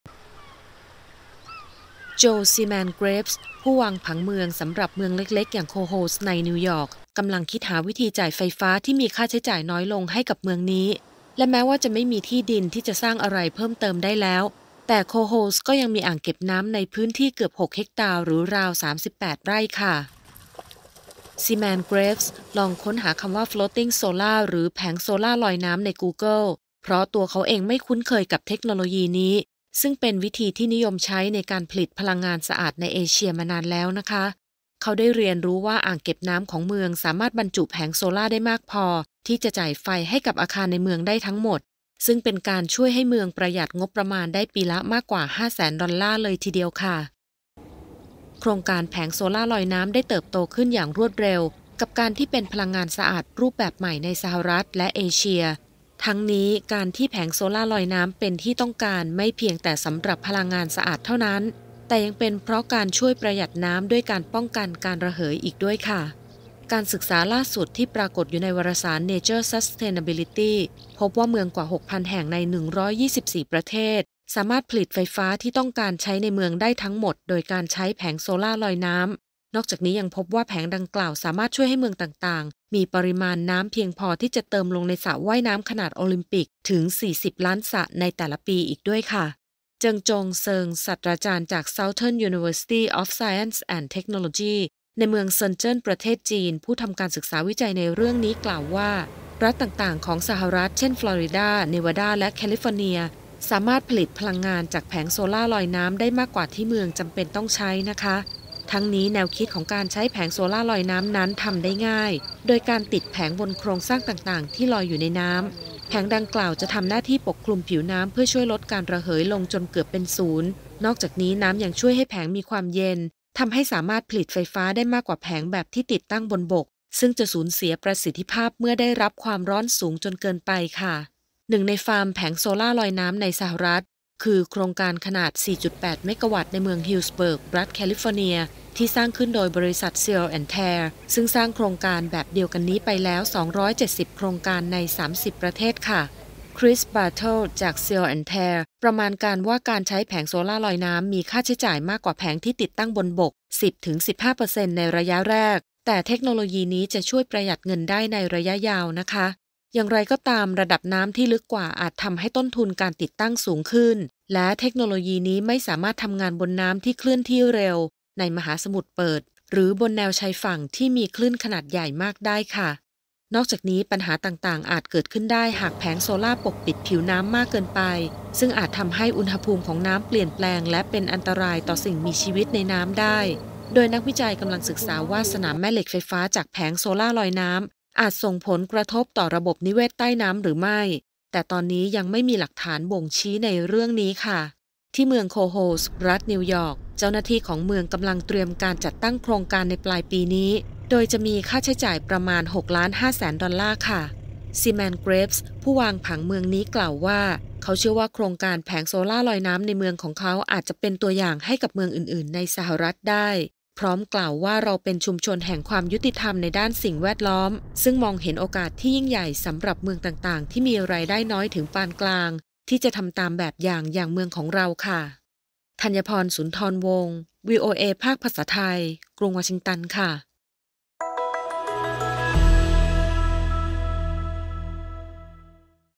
เทรนด์แผงโซลาร์เซลล์ลอยน้ำเริ่มติดกระแสในสหรัฐฯ
ขณะที่ ประเด็นการหาพลังงานหมุนเวียนหรือพลังงานทดแทนมาใช้ในชีวิตประจำวันยังคงเป็นสิ่งที่หลายฝ่ายพยายามเดินหน้าหาสิ่งที่ดีที่สุดให้ตนอยู่อย่างต่อเนื่อง และล่าสุด มีการศึกษาที่พบว่า คนในสหรัฐฯ กำลังเริ่มสนใจการใช้พลังงานแสงอาทิตย์ผ่านแผงโซลาร์เซลล์ในน้ำมากขึ้นเรื่อย ๆ แล้ว (ที่มา : เอพี)